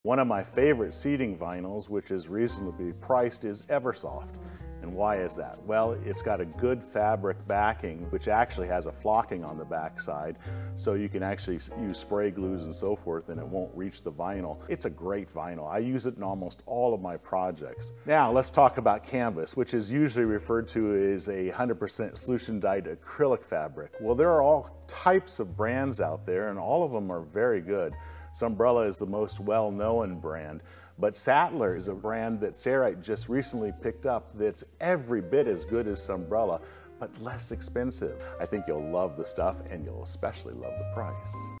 What's the best Seating Vinyl & Canvas Fabric?
Looking for the best fabric for your next project? Look no further than Eversoft Vinyl and Sattler Marine Canvas! These high-quality vinyl and canvas fabrics are perfect for all your DIY and sewing needs. Don't settle for anything less than the best when it comes to your projects.